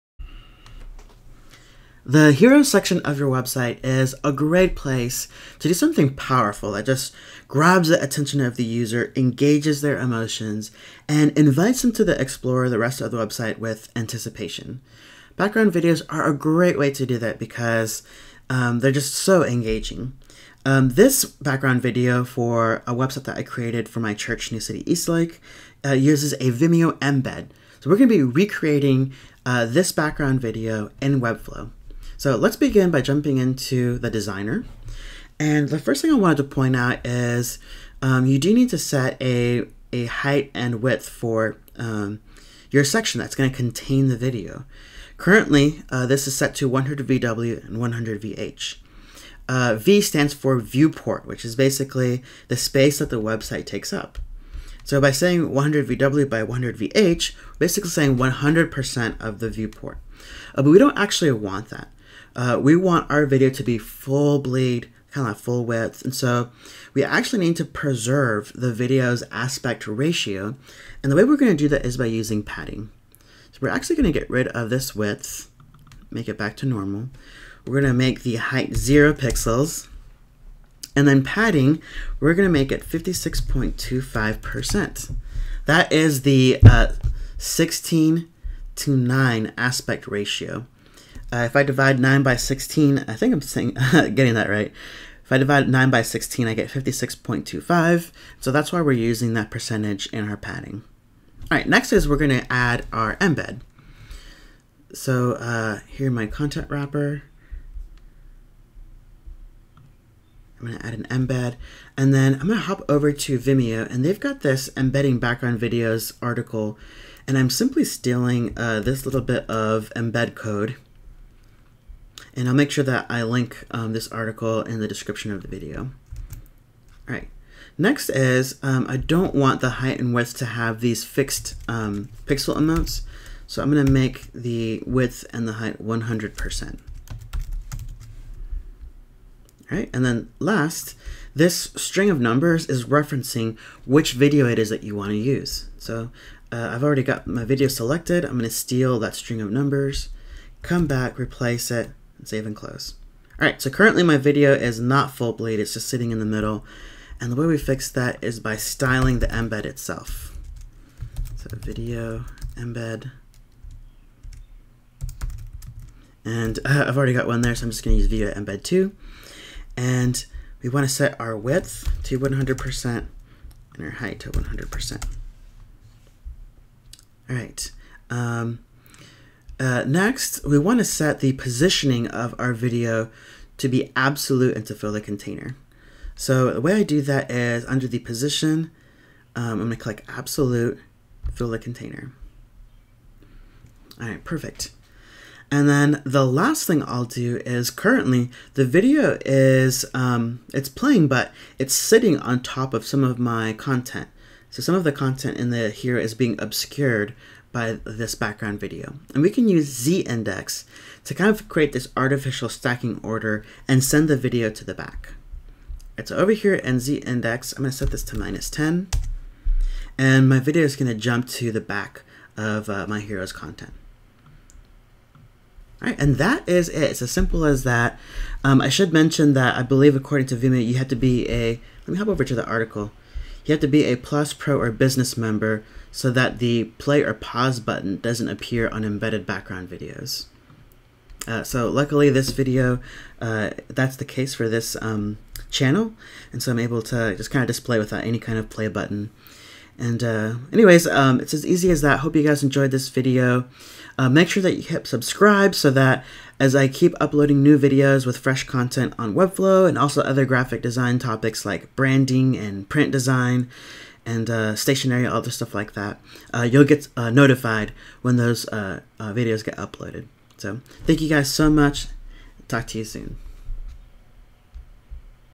Using a Vimeo Embed as a Background Video in Weblow
In this 5 minute video, learn how to use a Vimeo embed as the background video of a website developed in Webflow.